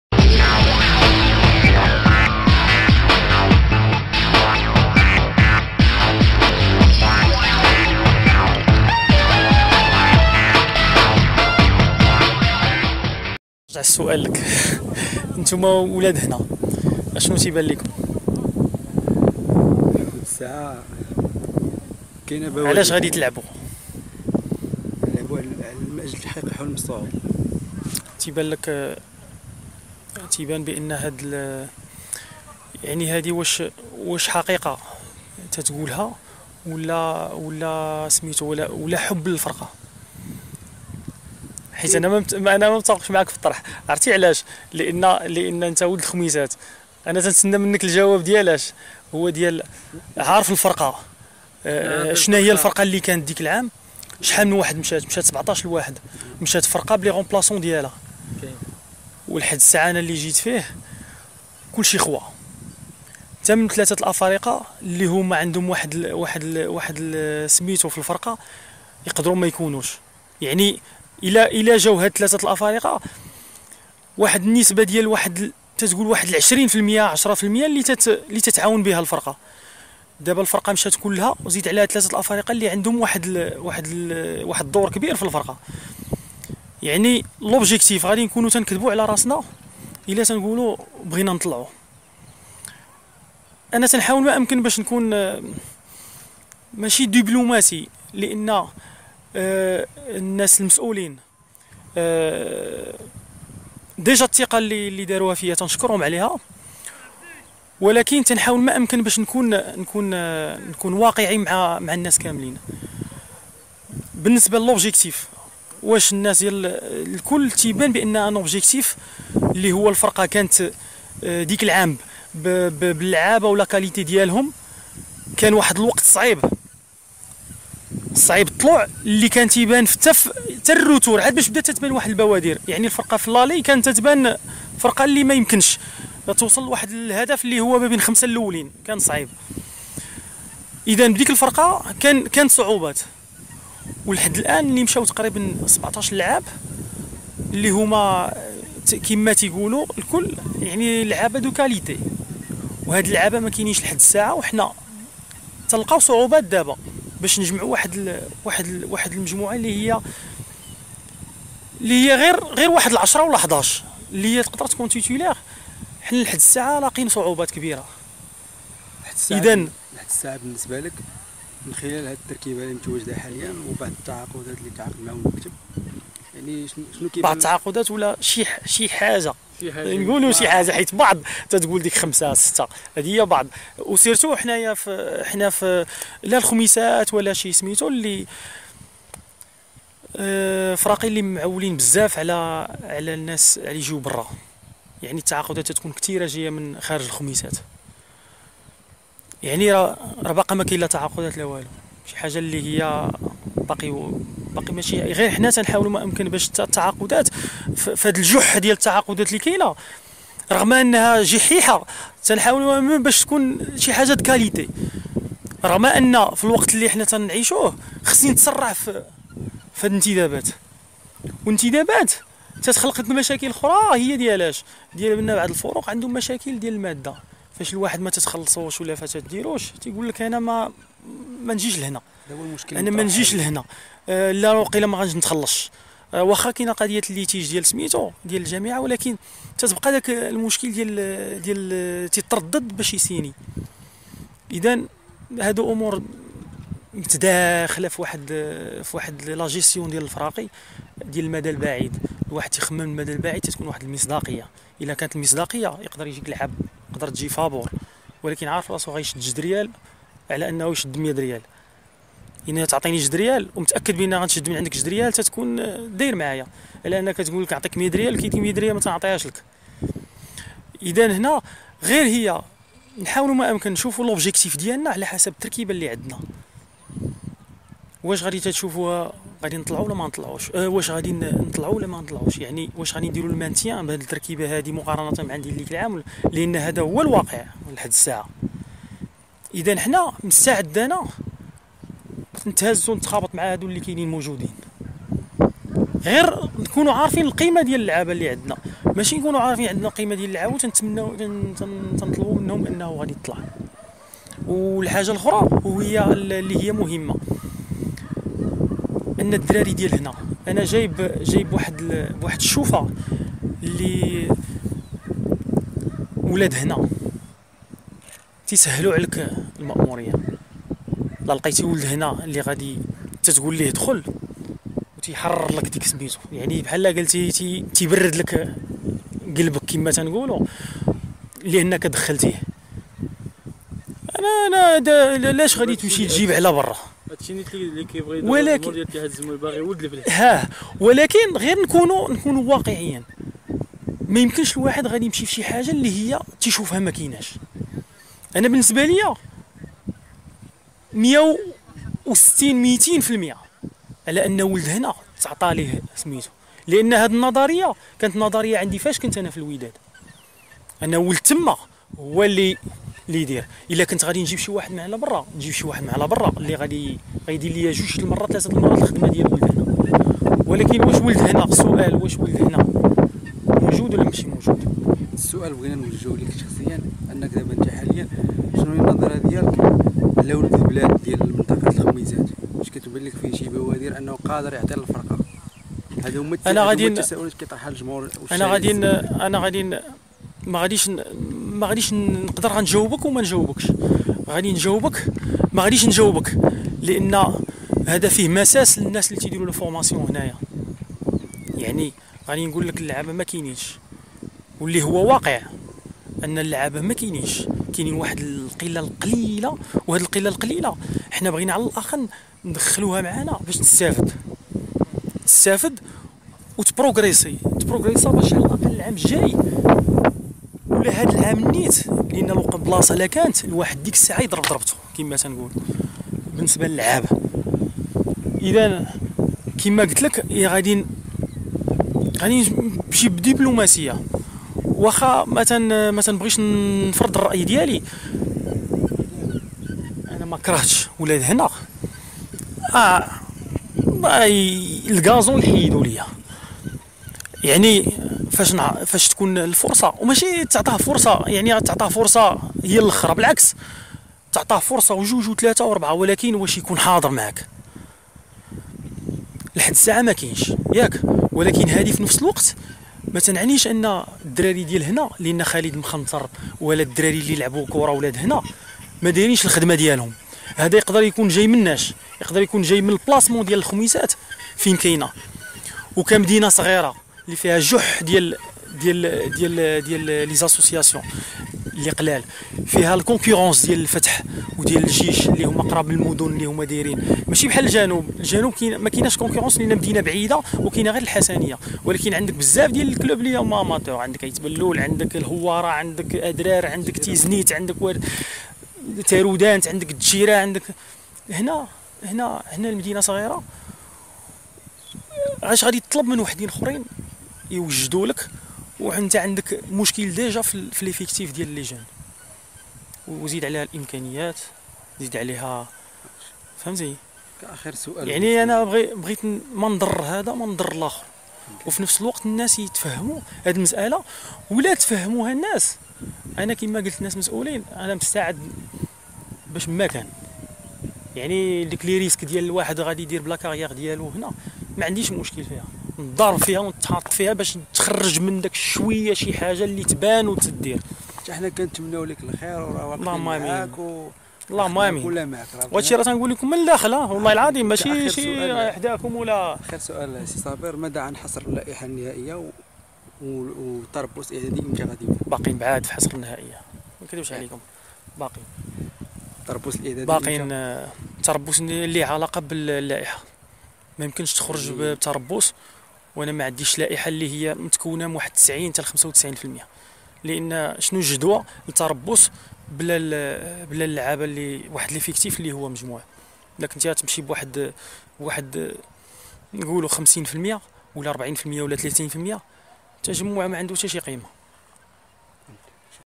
حوار حصري مع المدرب الجديد للكتيبة الزمورية عبد الإله صابر حول واقع الفريق قبل انطلاق المنافسات
الاطار الوطني عبد اللاله صابر مدرب فريق الاتحاد الزموري يفتح قلبه في حوار حصري عقب نهاية المباراة الاعدادية الرابعة امام النصر الفاسي ، حول واقع  ومستجدات الفريق الزموري قبل انطلاق منافسات البطولة الاحترافية اتصالات المغرب لاندية القسم الثاني .